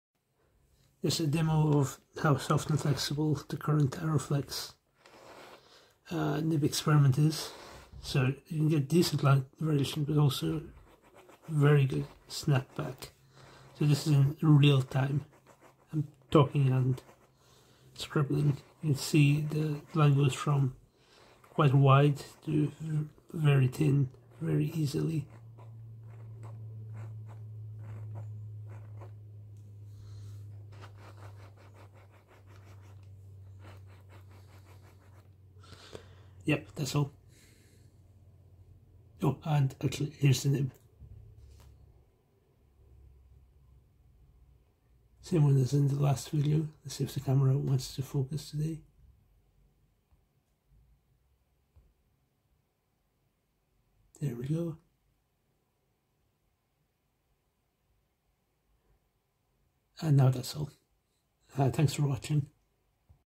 Real Time Arrowflex Softness and Snapback Demo
A jinhao fountain pen nib I modded to flex